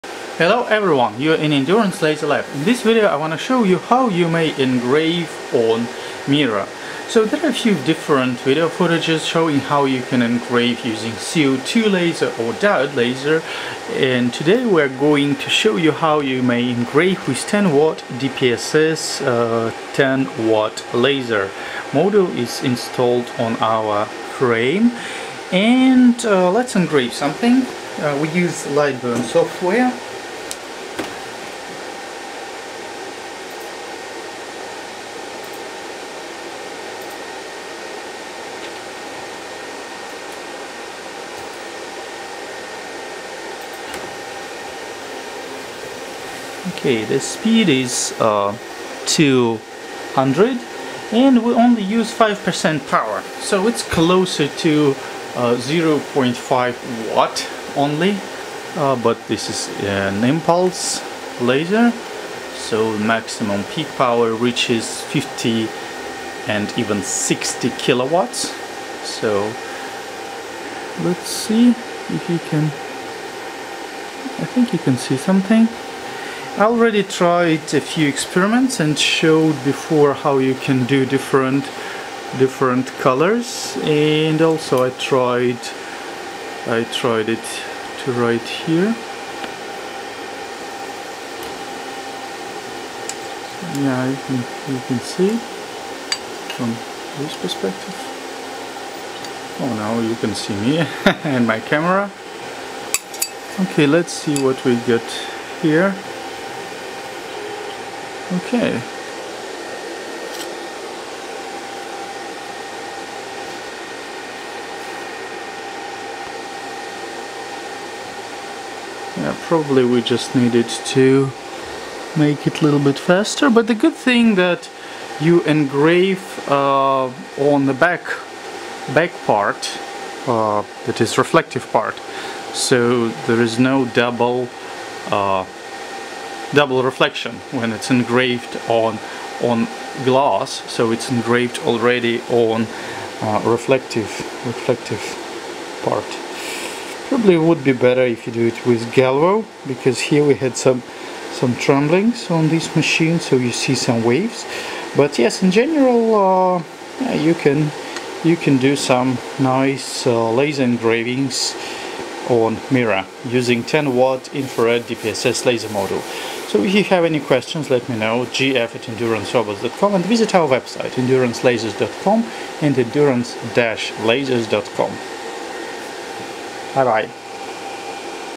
Engraving on a mirror using a Nd:YAG 1064 nm impulse laser - a full video guide
It is very easy to engrave on a mirror using an infrared 1064 nm impulse DPSS laser attachment